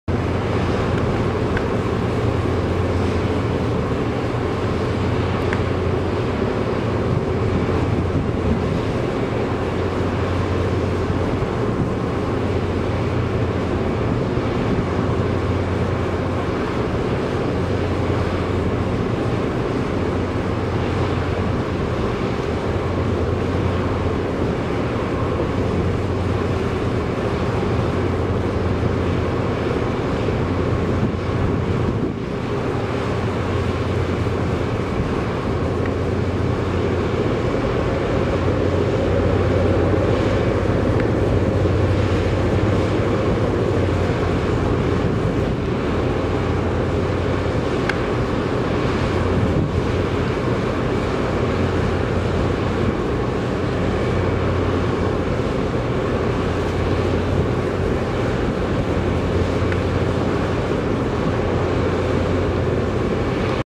PyroBoom Burns in Gulf of Mexico
AFTI supplies PyroBoom, a specialized form of fire boom, to assist with clean-up efforts in the Gulf of Mexico. This is an example of in situ burning being widely used in clean up efforts.